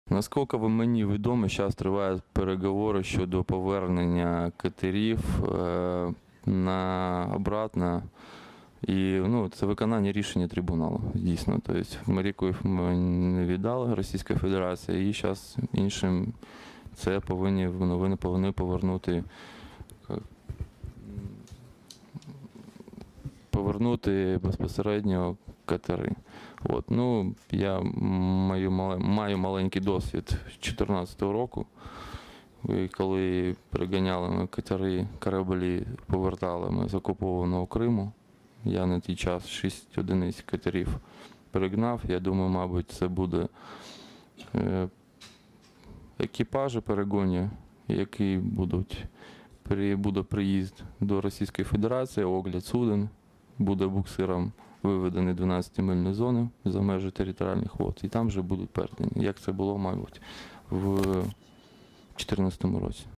Звільнені моряки: як і коли можуть повернути українські катери
Капітан другого рангу Денис Гриценко розповів про те,як може відбутися повернення українських військових катерів "Бердянськ", "Нікополь" і буксиру "Яни Капу"
Звільнені з російського полону моряки провели пресконференцію в Укрінформі.
25 листопада 2018 року російські силовики біля берегів анексованого Криму обстріляли і захопили три кораблі Військово-морських сил України: катери "Бердянськ", "Нікополь" і буксир "Яни Капу", які прямували з Одеси до Маріуполя. У полон потрапили 24 українці, троє з них були поранені. Підконтрольний Росії суд в окупованому Криму заарештував всіх затриманих українських військових за обвинуваченням у нібито незаконному перетині